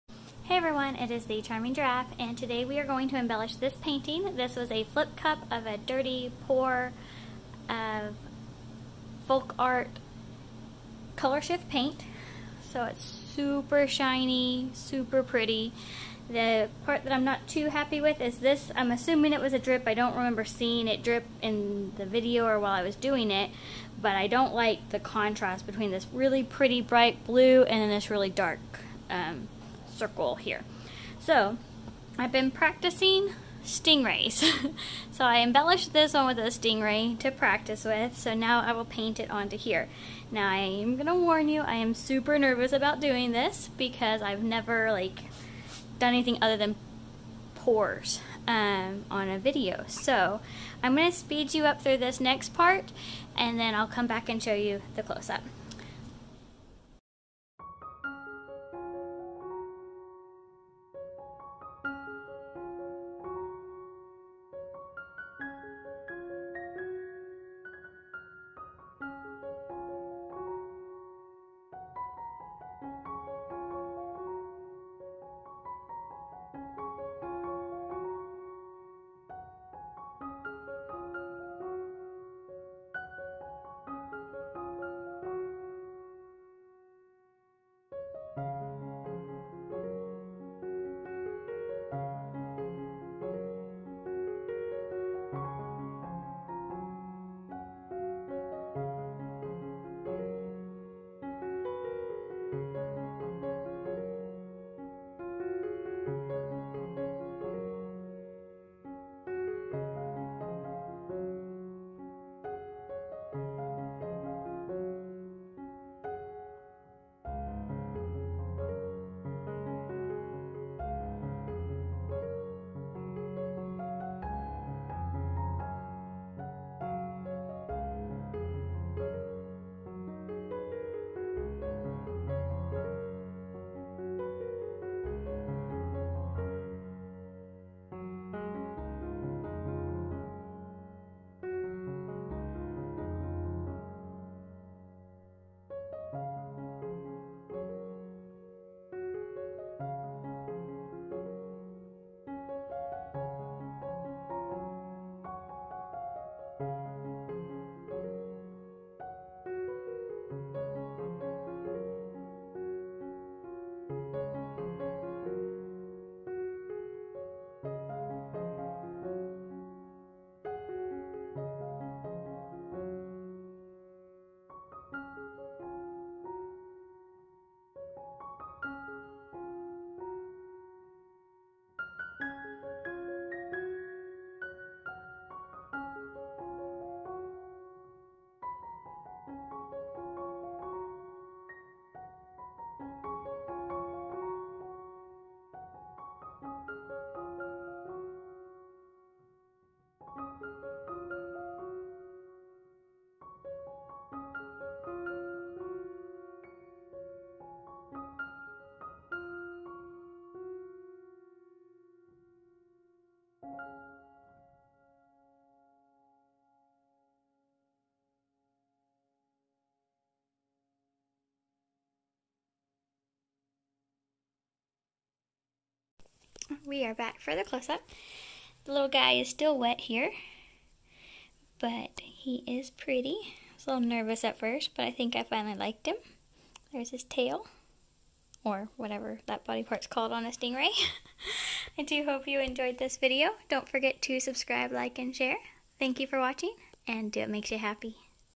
Embellishing an Acrylic Pour Painting with a Stingray YT100 Day 44
In this video I embellish an acrylic pour painting by painting a stingray on it!

Materials used:
-Acrylic pour painting
-Paint
-Paintbrush

Paint ratio approximates:
-70% floetrol, 30% paint, then water to consistency if necessary (amount of water varies based on paint type)